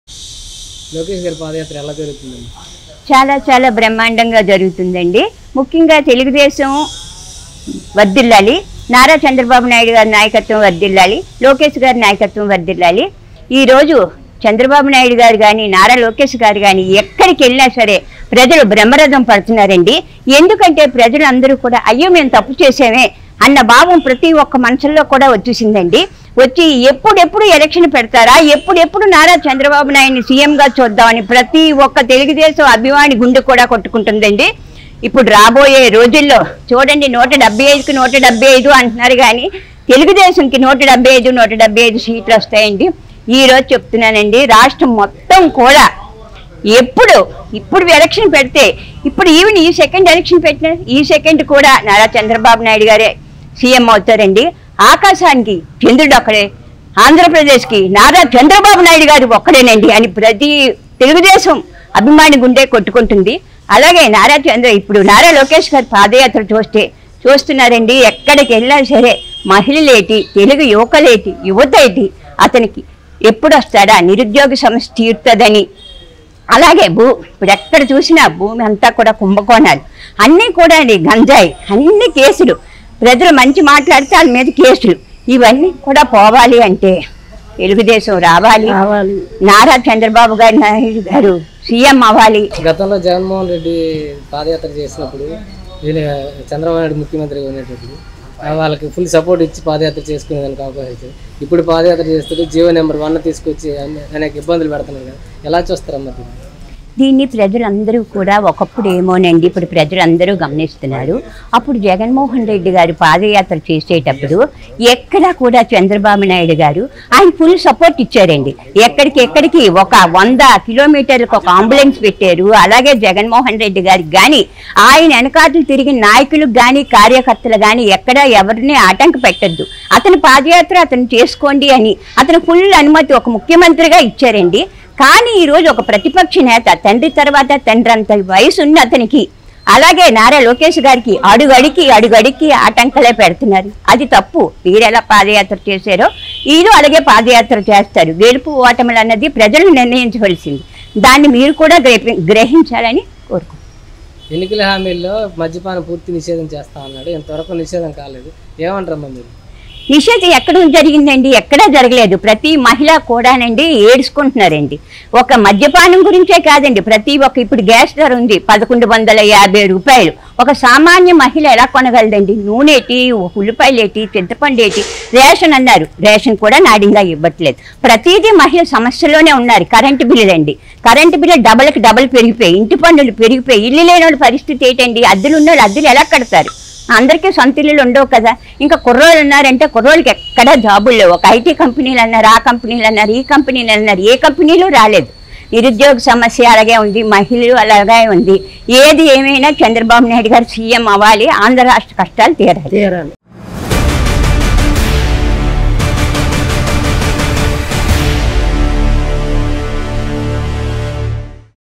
TDP Woman Activist Slams on Jagan Ruling | Ap Elections 2024 | Ys Jagan | Public Talk | Open Talk
Category
News & Politics